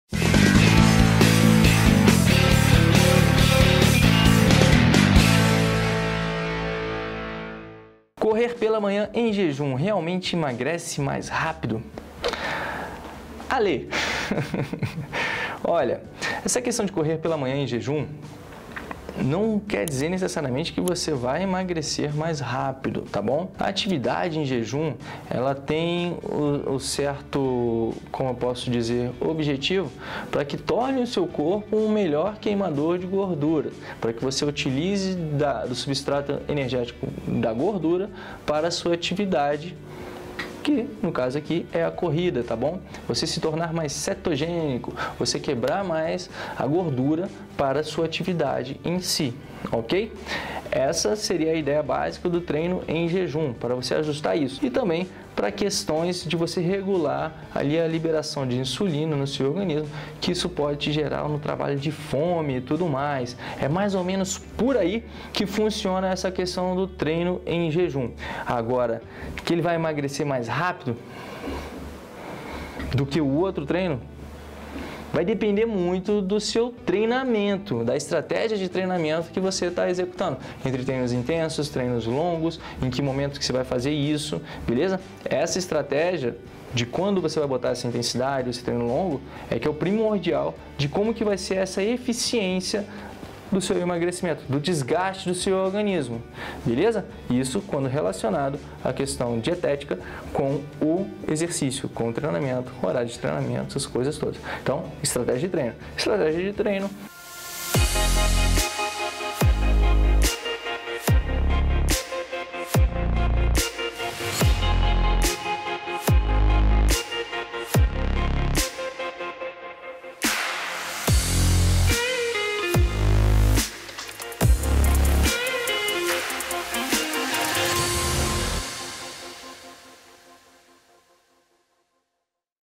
Corrida em JEJUM: será que essa é a MELHOR estratégia para EMAGRECER rápido?
Será que correr em jejum emagrece mais rápido? Nesse vídeo você encontra a resposta dessa e outras dúvidas sobre emagrecimento e corrida.

📲 Baixe GRÁTIS nosso aplicativo para acessar exercícios que vão melhorar sua corrida para você emagrecer: